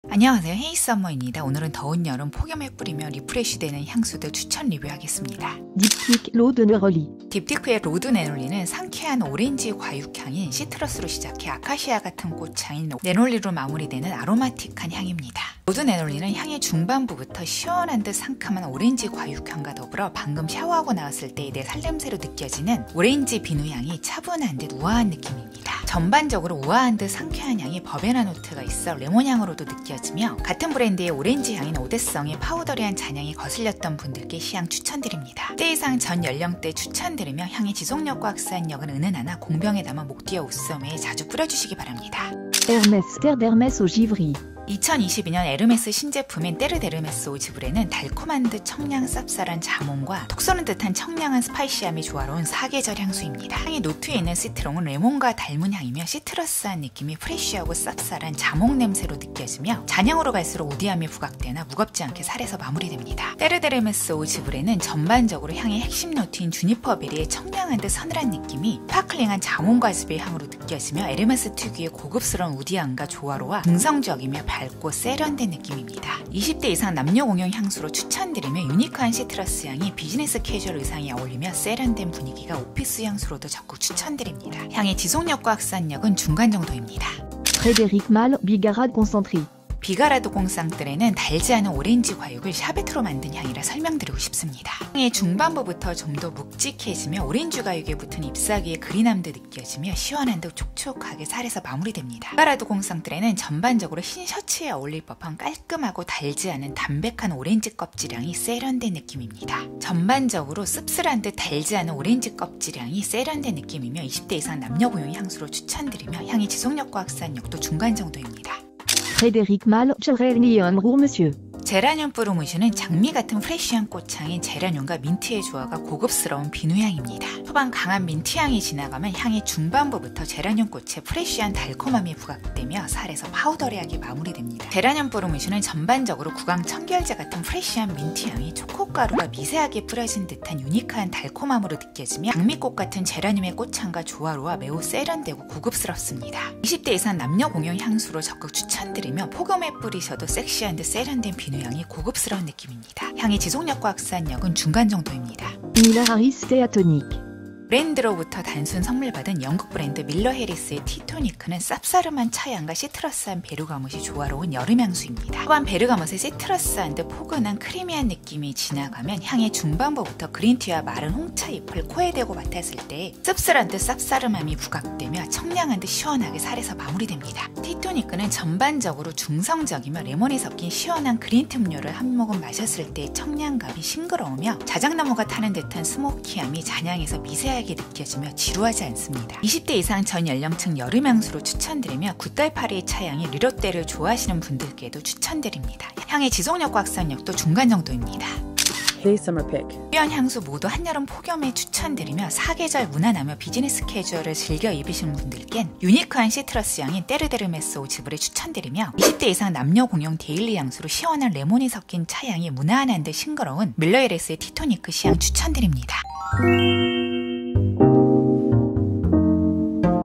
폭염에 뿌리는 향수(딥디크,에르메스, 프레데릭말,밀러해리스)feat 에르메스 2022년 신상👍🏻
✔️Diptyque 00:05
L'Eau de Neroli
로 드 네롤리

✔️Hermès 00:42
Terre d'Hermes Eau Givree
떼르 데르메스 오 지브레 edp

✔️Frederic Malle 01:27
Bigarade concentree
비가라드 꽁쌍드레

✔️Frederic Malle 01:58
GERANIUM ROUR MONSIEUR
제라늄 뿌르무슈￼

✔️(Brand Gift)
Miller Harris  02:35
Tea Tonique
밀러해리스 티토니끄

📌 정리:Pick 03:18